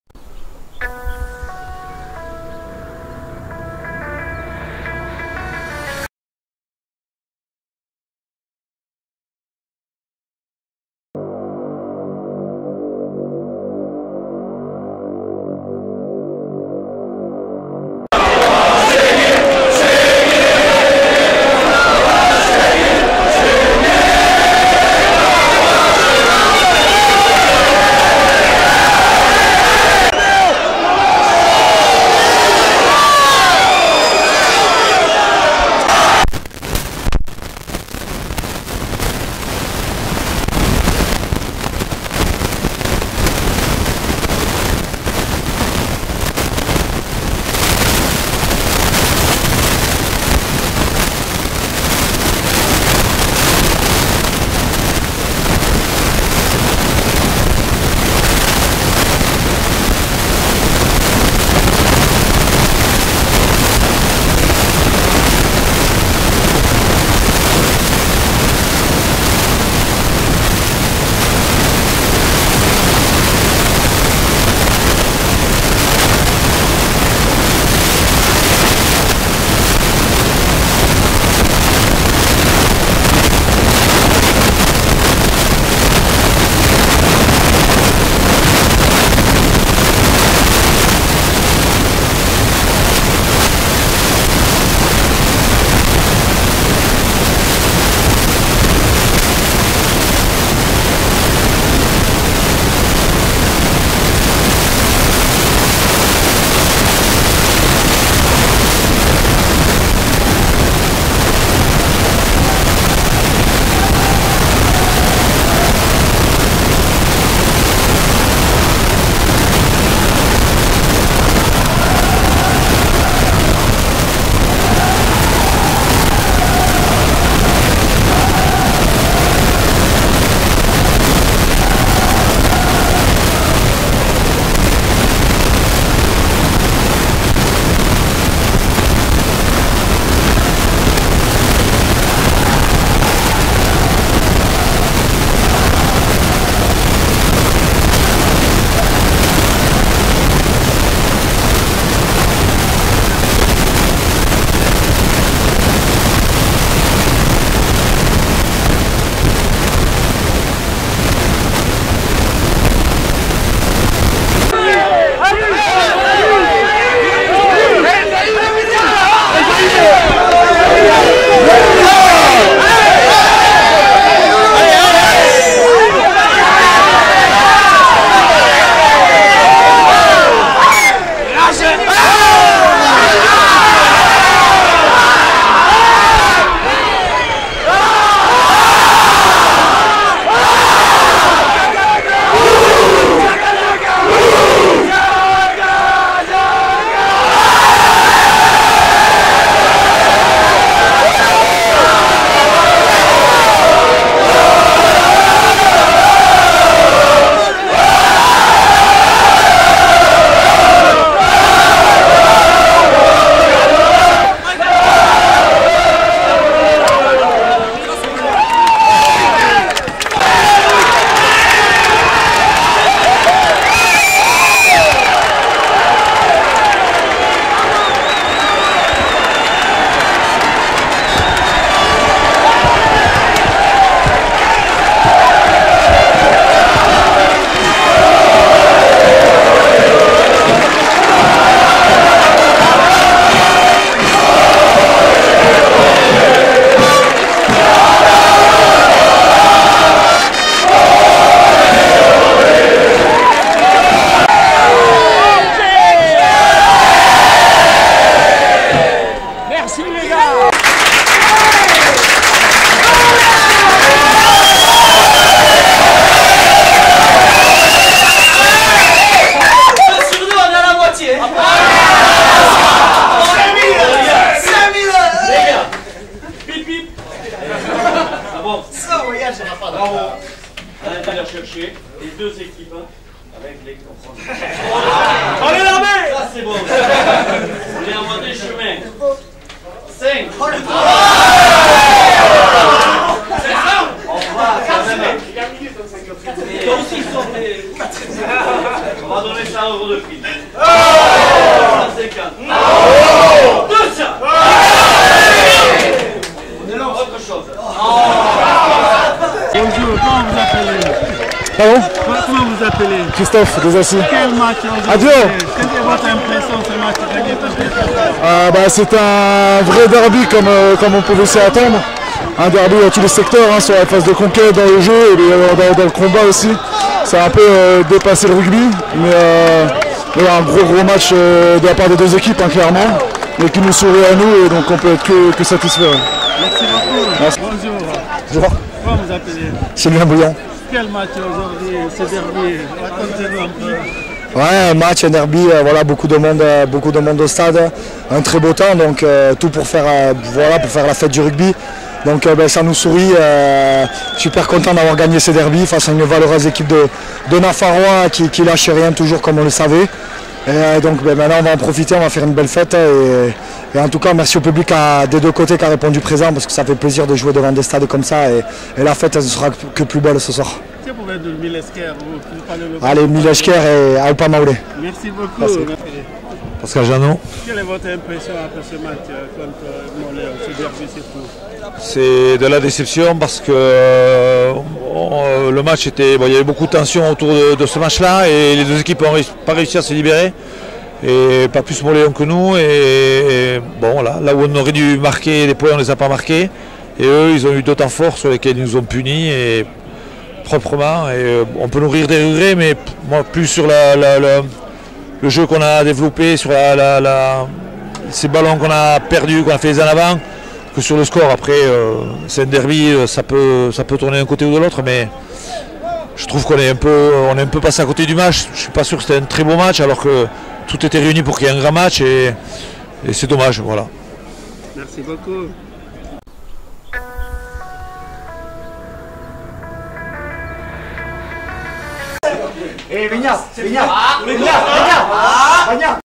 Mauleon explose de joie après la victoire contre Nafarroa
Le derby Basque
Mauleon contre Nafarroa